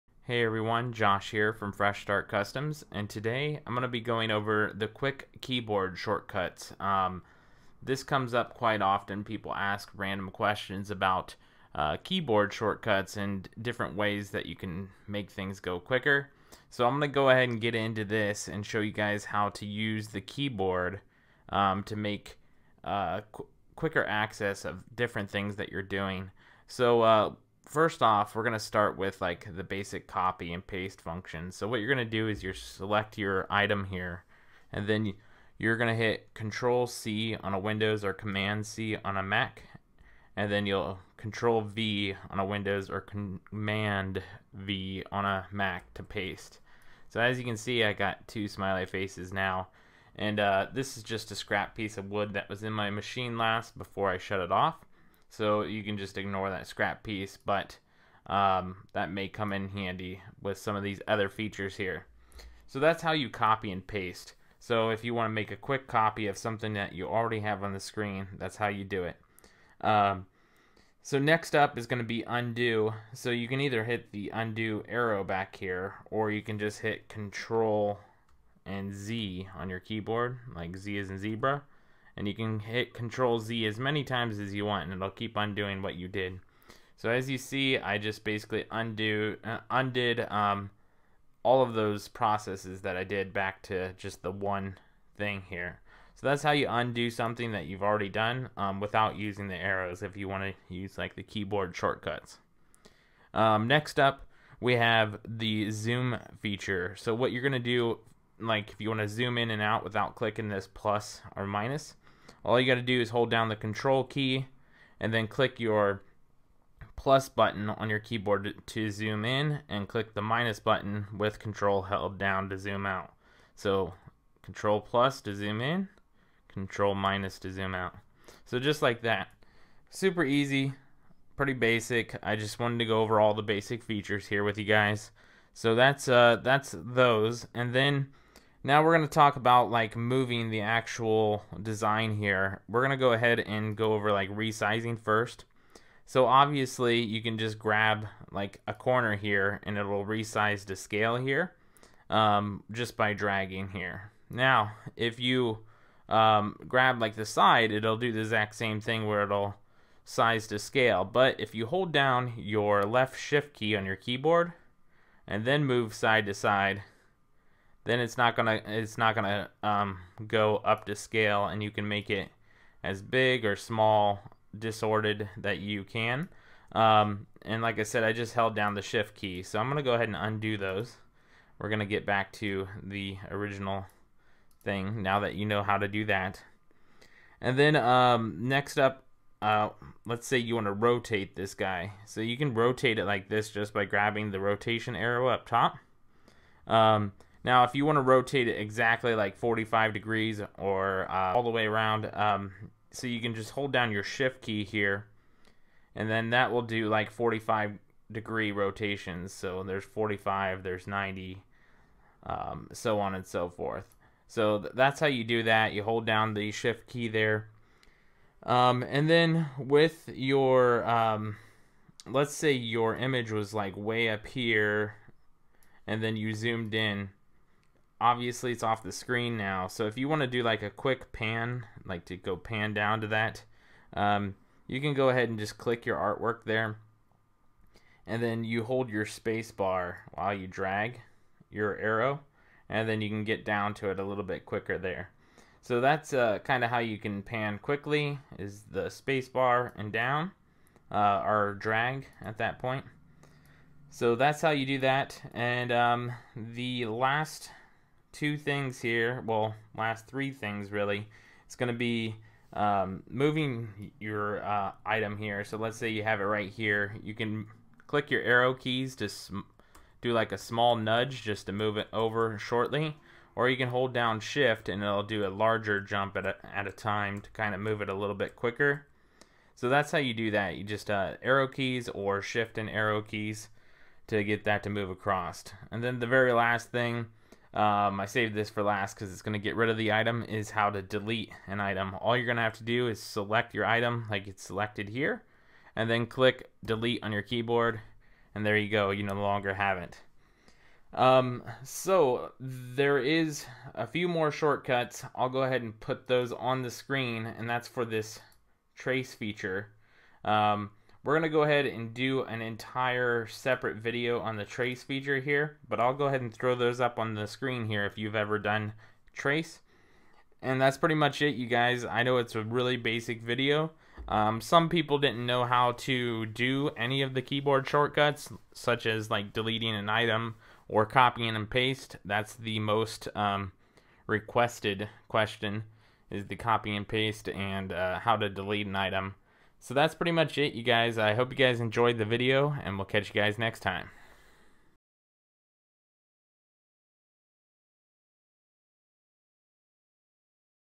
How to use Glowforge Shortcuts
Quick video going over the shortcuts in Glowforge UI

Our custom laser file designs can be found in my Etsy shop